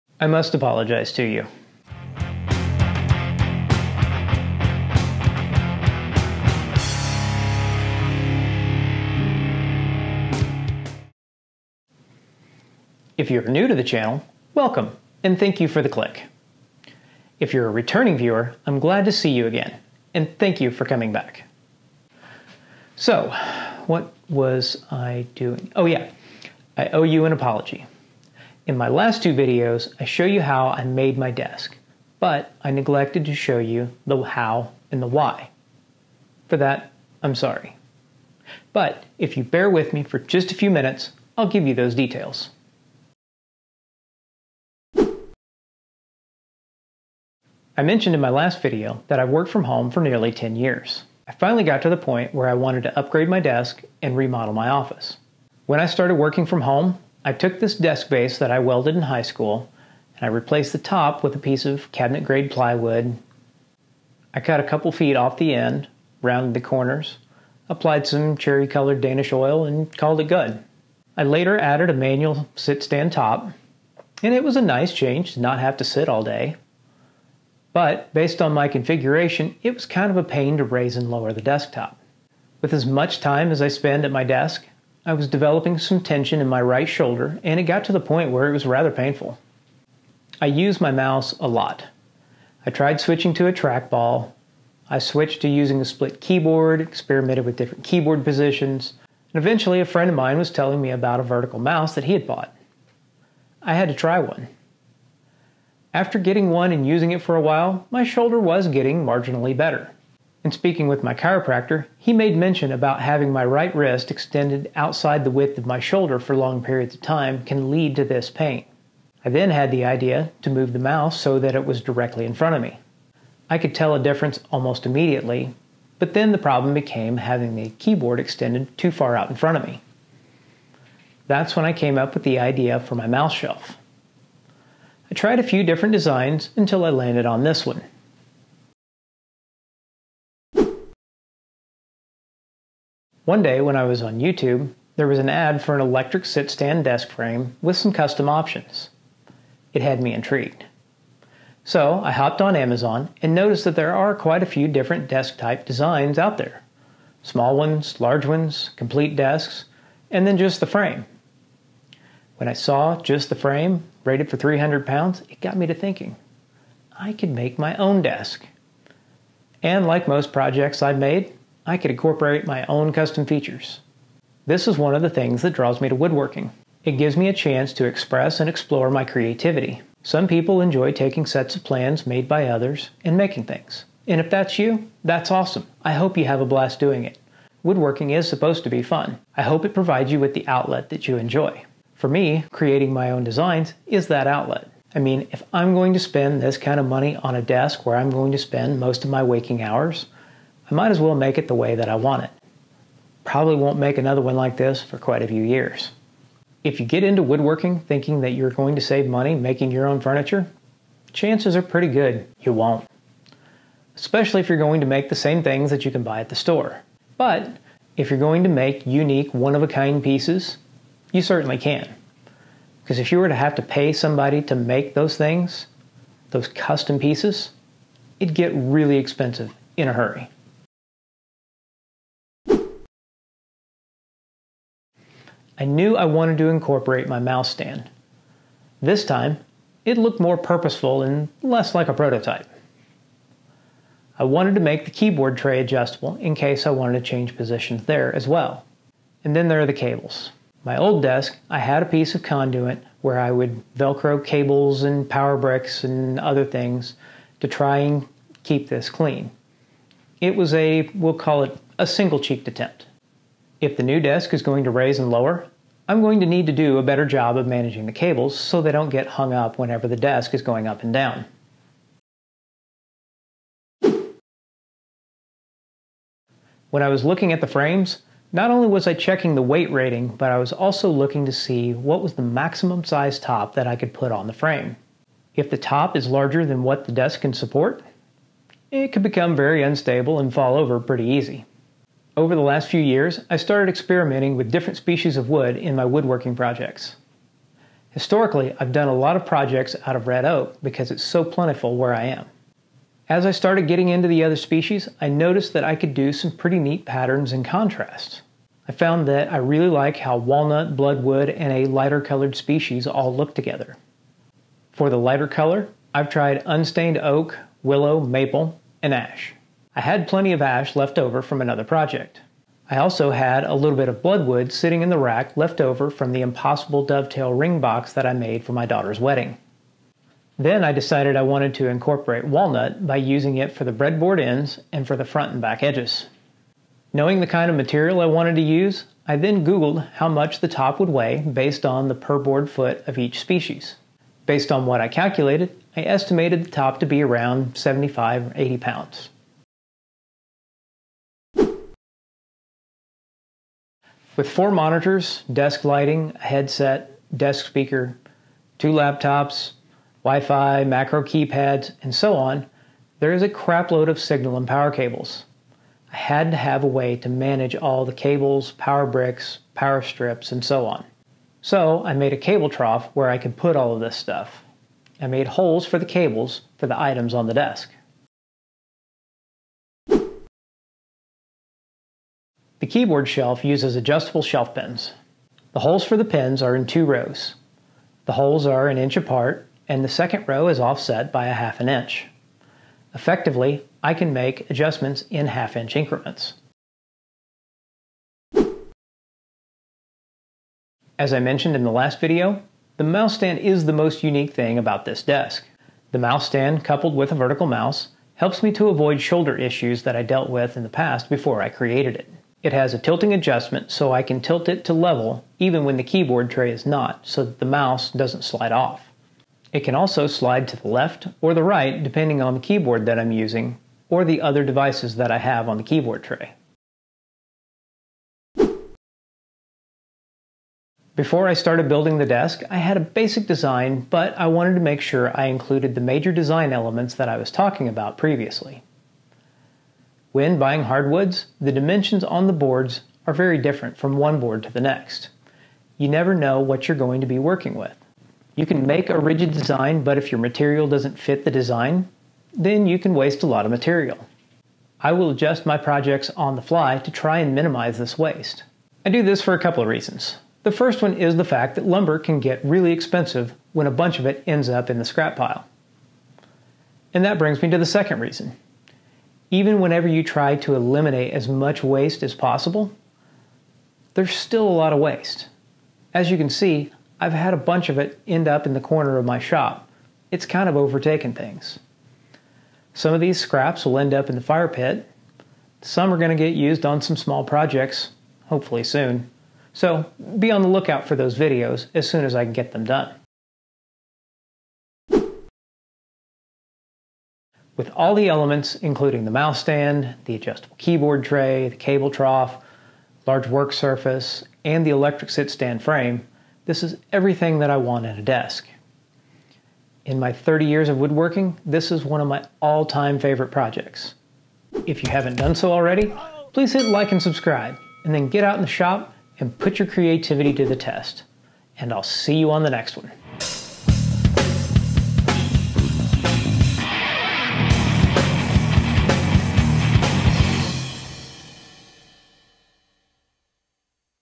Design the ultimate standing desk
In this exciting three-part woodworking series, join me as I take you through the process of creating my ultimate electronic ergonomic sit-stand desk with built-in cable management. This desk, affectionately dubbed "Franken Desk" due to its rich history of multiple iterations, has evolved over nearly 30 years of tinkering.

Thank you for joining me on this woodworking journey, and I've enjoyed sharing the completion of "Franken Desk" with you!